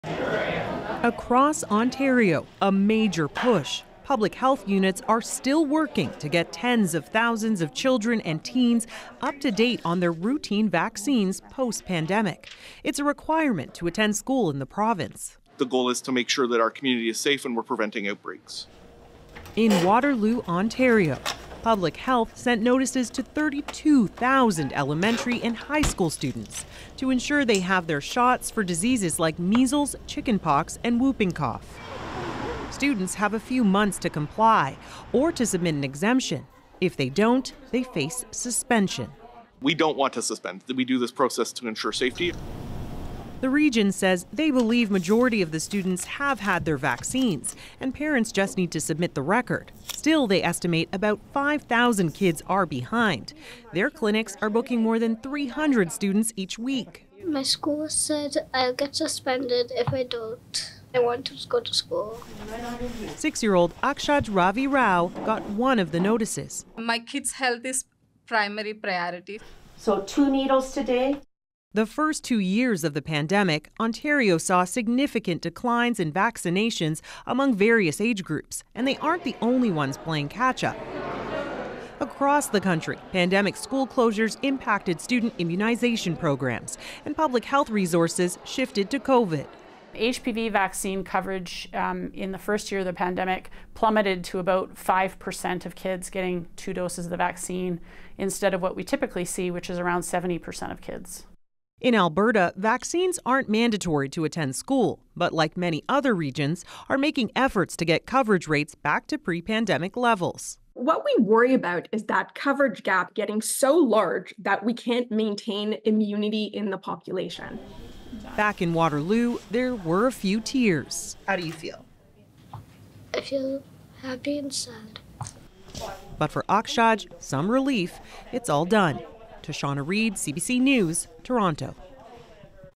Ontario issues warning: update kids’ vax records or face school suspension
Tens of thousands of Ontario students are facing suspension if they don't update their vaccination records, as the province pushes hard to get kids caught up on shots for everything from COVID-19 to chickenpox.